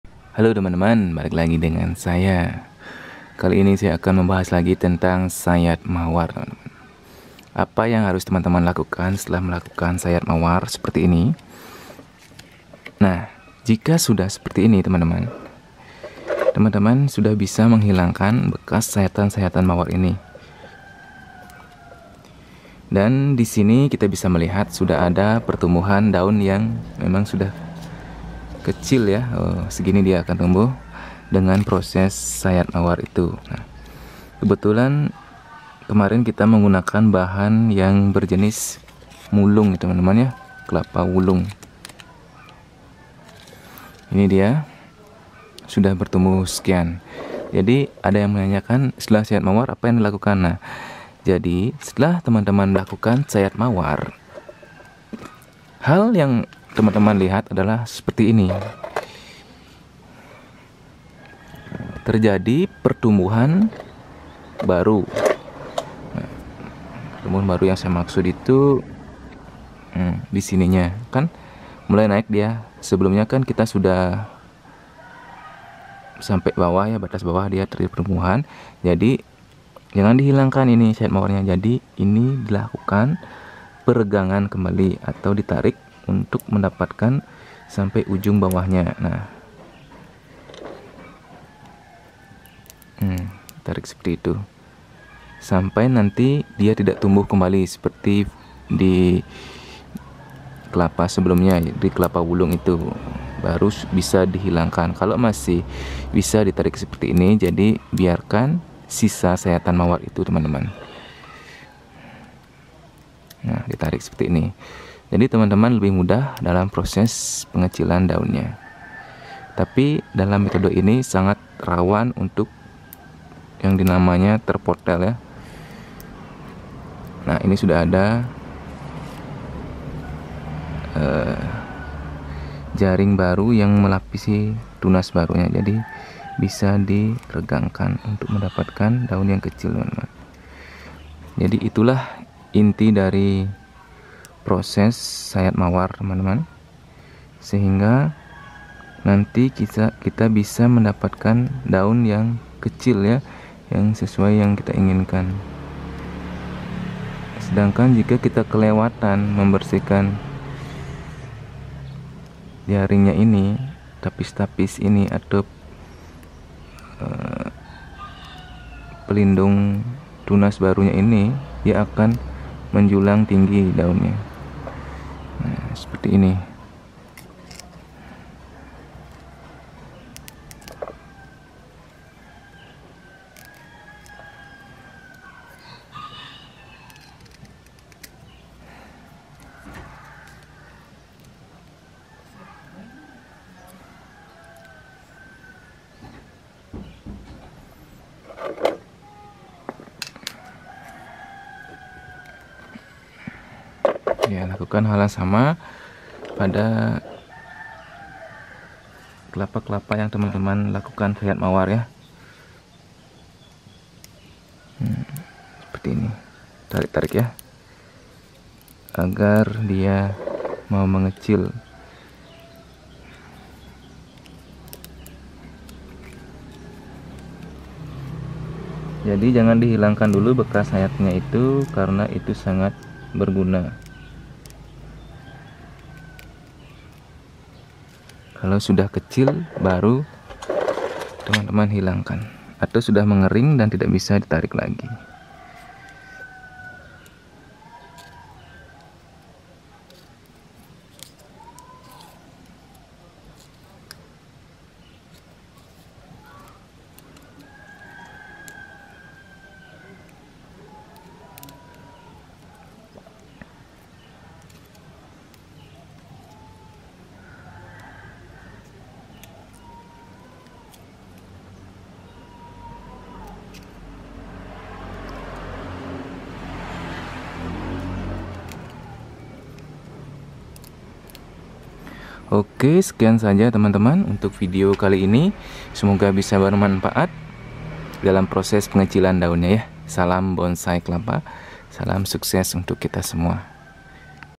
HAL YANG HARUS DILAKUKANS SETELAH SAYAT MAWAR BONSAI KELAPA
sayat mawar cara mengkerdilkan daun sejak awal, dan proses ini membutuhkan sebuah kesabaran

SIMAK JUGA